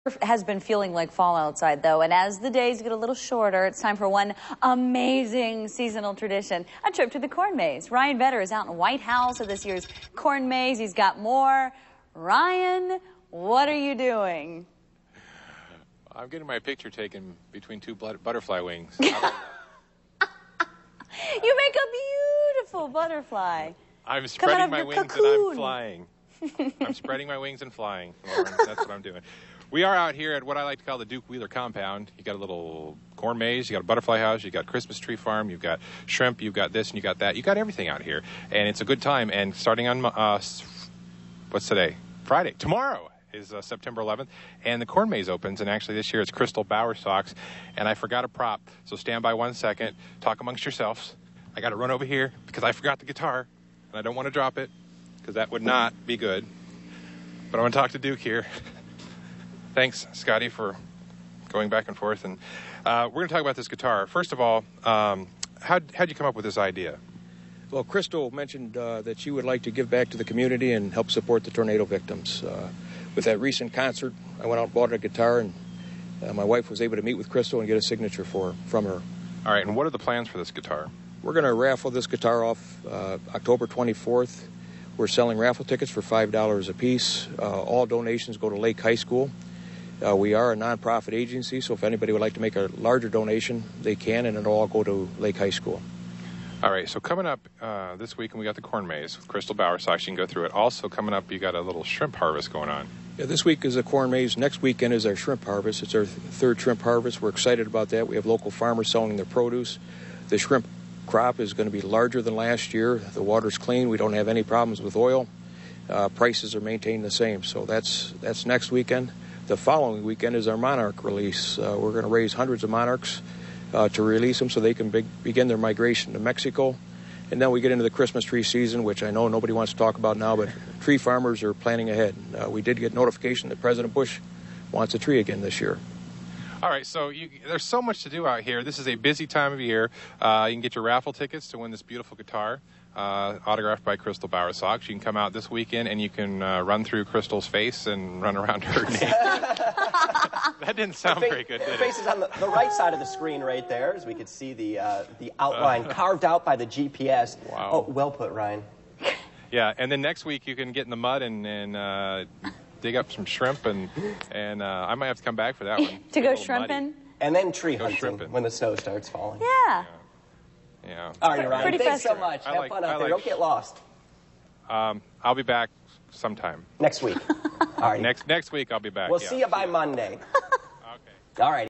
Corn Maze 2
Talking corn maze, butterflies and shrimp this morning in Whitehouse, Ohio. Also talking about a raffle for a guitar autographed by Crystal Bowersox.